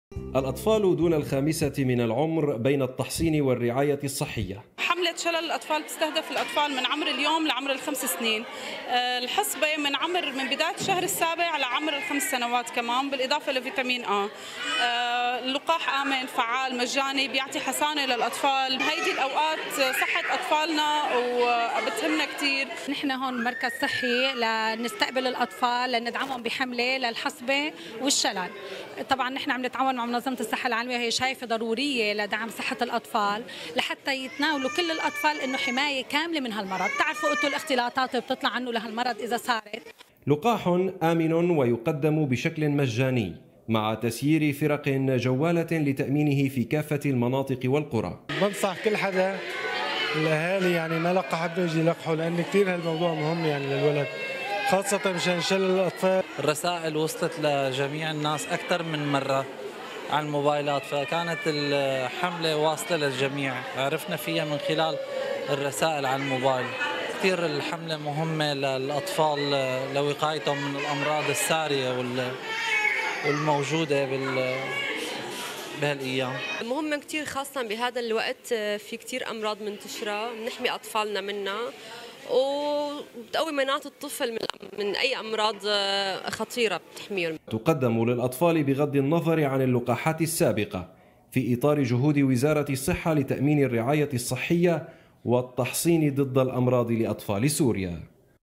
اللاذقية - استمرار الحملة الوطنية للتلقيح ضد شلل الأطفال والحصبة
تقرير: مازن محمد